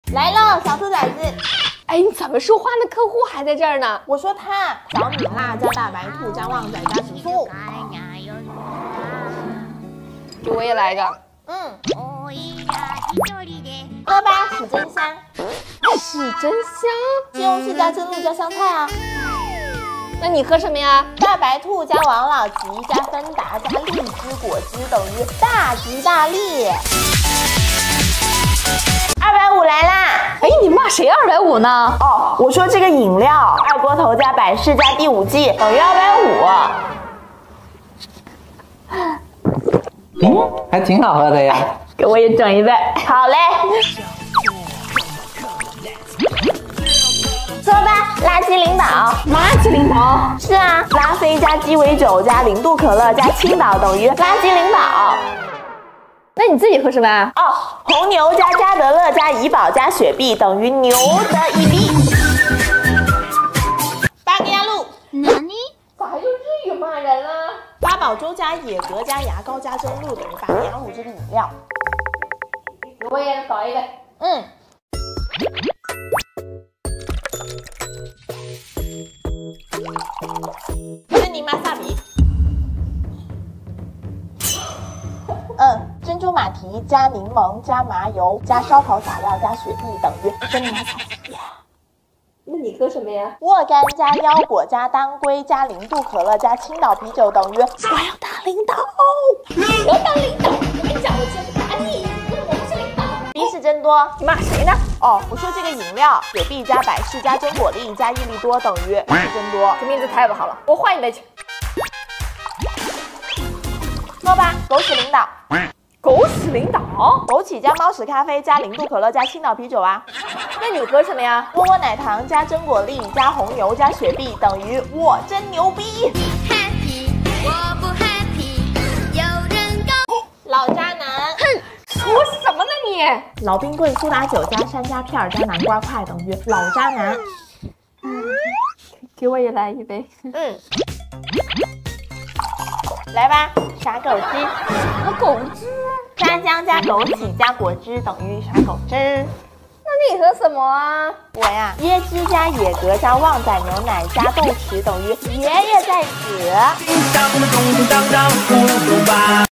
週總想喝飲料，菌菌立馬安排一杯“小兔崽子”，沒想到領導卻想喝‘’史珍香‘’！ 【小菌想升職】
大家好，我是小菌
辦公室裡的新鮮事
老闆和員工之間的愛恨情仇
我這裡都有😎😎😎
持續輸出搞笑內容，讓你每天哈哈哈